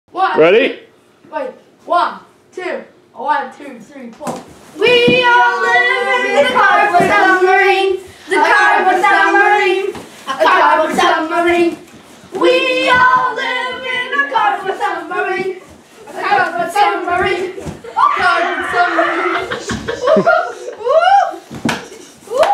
The Cardboard Submarine - Completed!
And it's done. The boys have a song to celebrate the completion!  I think they like it.